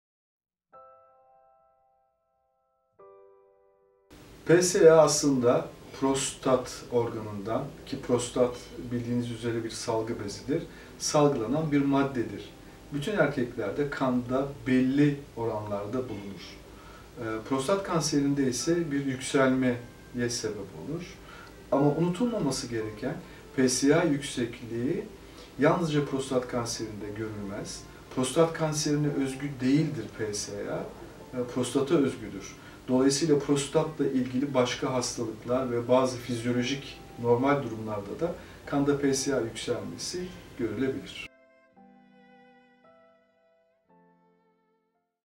PSA nedir? - Prof. Dr. Süleyman Ataus
Prof. Dr. Süleyman Ataus “PSA nedir?” sorusunun yanıtını veriyor.

PSA aslında bir salgı bezi olan prostat organından salgılanan bir maddedir. Bütün erkeklerde kanda, belli bir oranda bulunur. Prostat kanseri de bu oranda bir yükselmeye neden olur. Ancak unutulmaması gereken, PSA yüksekliği yalnızca prostat kanserinde görülmez.

PSA prostat kanserine özgü değil, prostata özgüdür. Dolayısıyla, prostatla ilgili başka hastalıklarda ve bazı fizyolojik normal durumlarda da kanda PSA yükselmasi görülebilir.